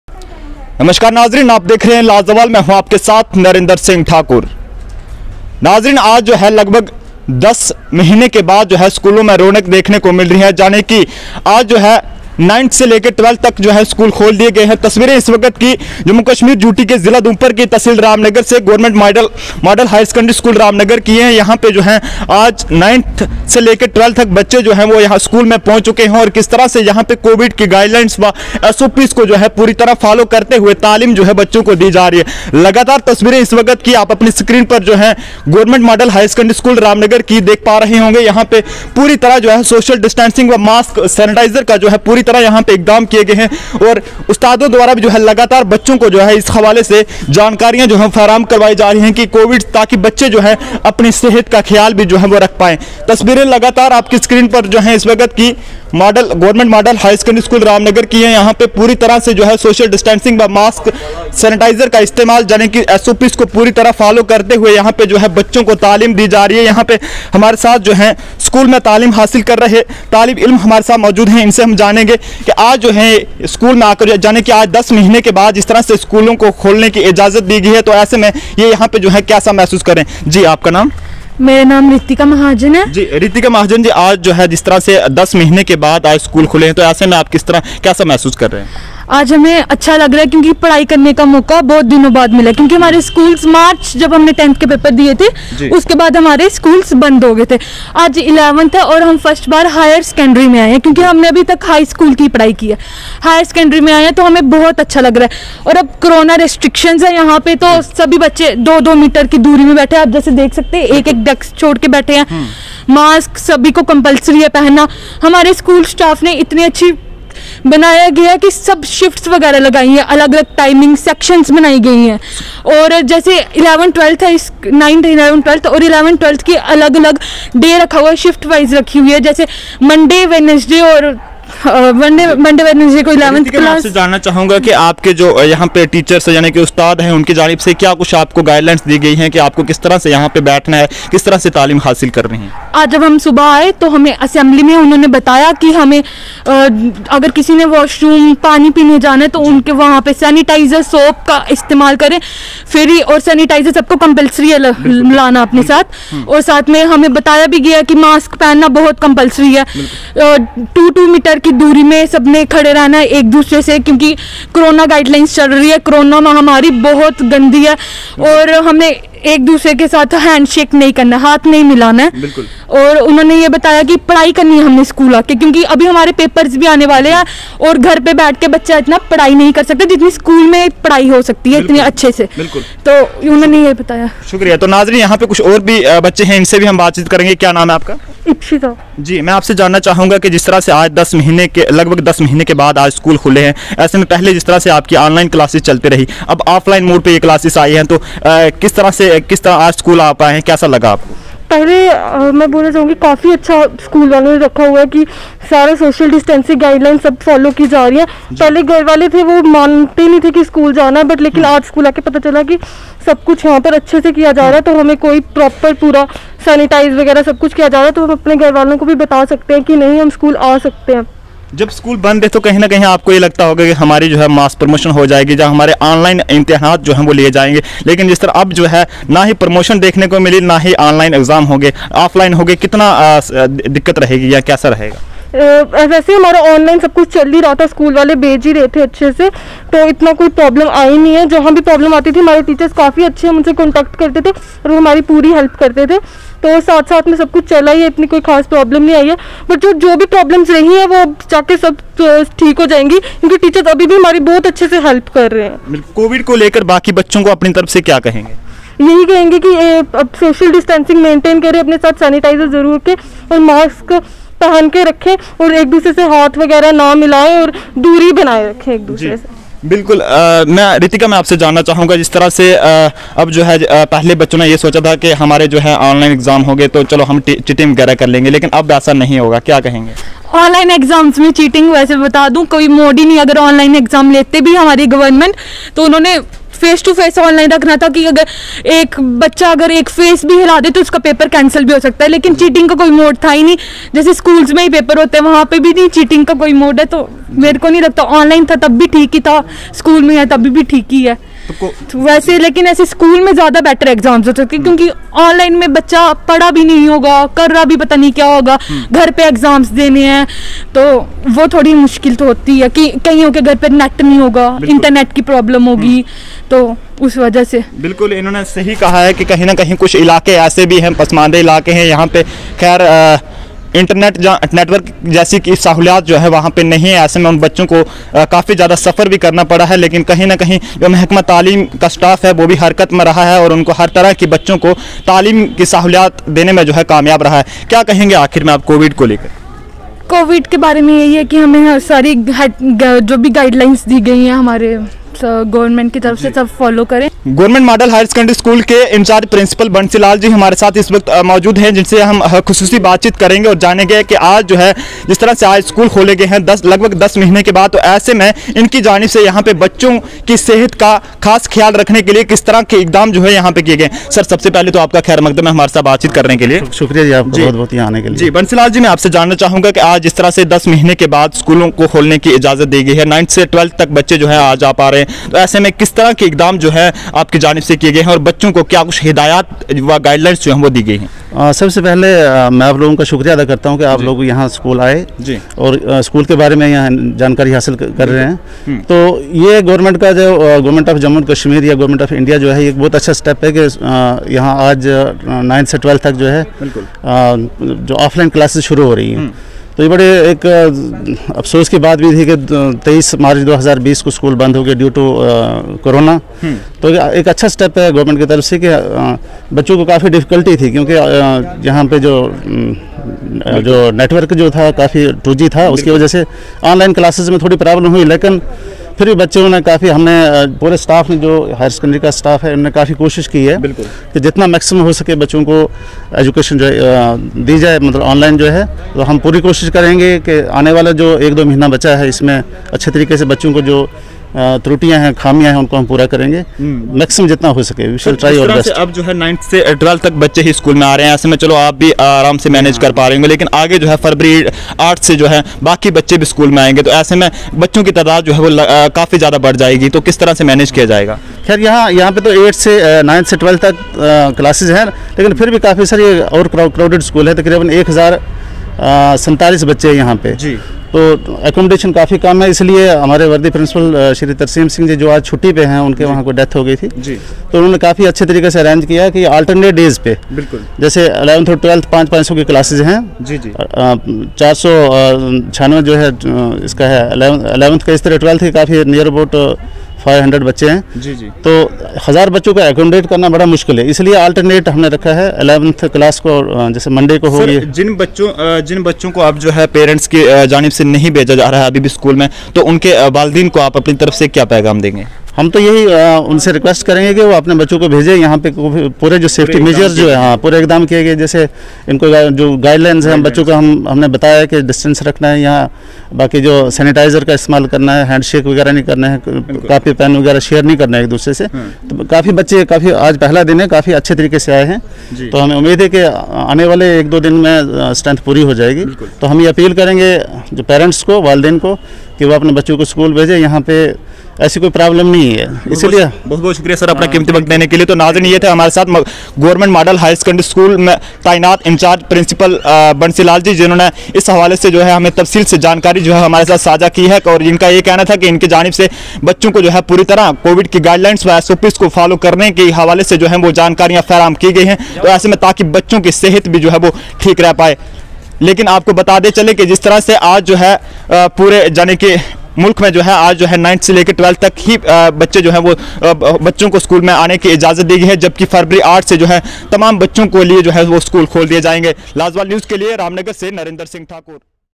||Padhai Hai Zaruri Magar Do Gaz Ki Duri Bhi Hai Zaruri|| 10 Maheene Baad Khul Gaye School, Bachu
||Padhai Hai Zaruri Magar Do Gaz Ki Duri Bhi Hai Zaruri||

10 Maheene Baad Khul Gaye School, Bachu Mai Padhayi Ka Josh aur Khushi Ki Leher
Govt Model Higher Secondary School Ramnagar